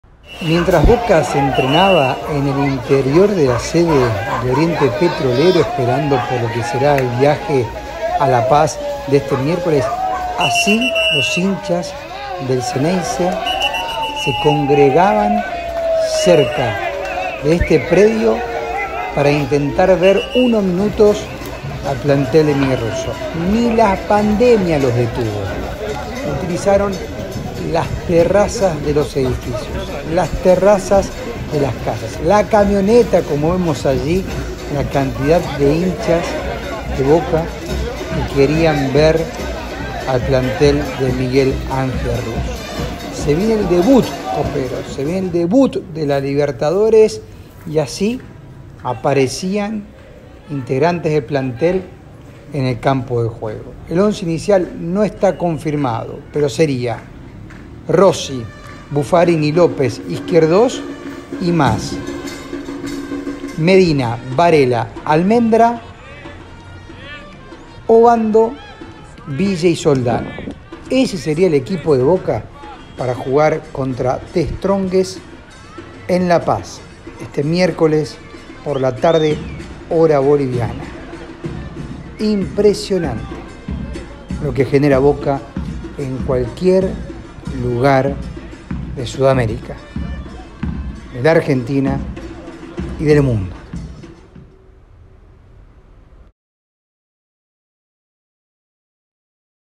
Locura por Boca Juniors en Bolivia. Miles de hinchas se acercaron a ver la práctica.
Boca Juniors se entrenó en el predio de Oriente Petrolero y mientras los jugadores del Xeneize se movían en una de las canchas, los hinchas usaron las Terrazas de los edificios, casas, las cajas de las camionetas, para poder ver lo que realizaban los futbolistas en el predio utilizado en la ciudad de Santa Cruz de la Sierra.
Una locura por Boca en Bolivia.